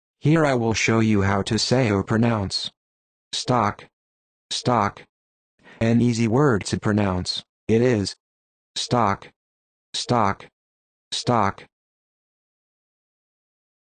How to Say or Pronounce Stock
This video shows you how to say or pronounce Stock.

How accurate does it say Stock? How would you say Stock?